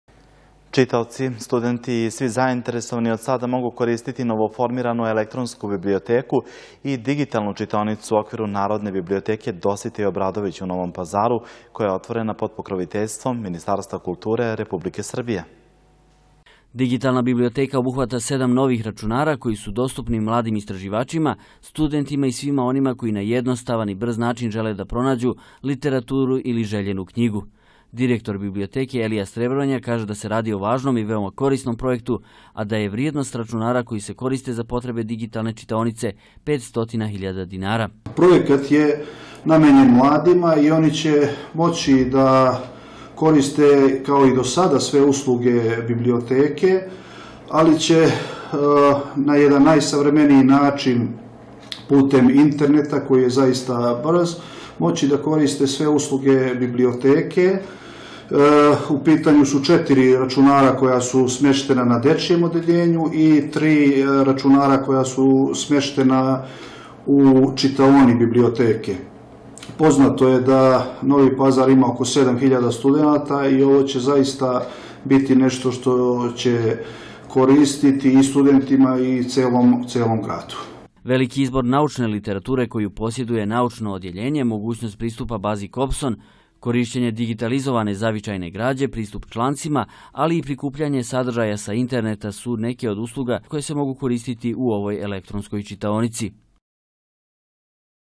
Otvorena elektronska čitaonica u Gradskoj biblioteci
Čitaoci, studenti i svi zainteresovani od sada mogu koristiti novoformiranu elektronsku biblioteku i digitalnu čitaonicu u okviru Narodne biblioteke “Dositej Obradović“ u Novom Pazaru koja je otvorena pod pokroviteljstvom Ministarstva kulture Republike Srbije.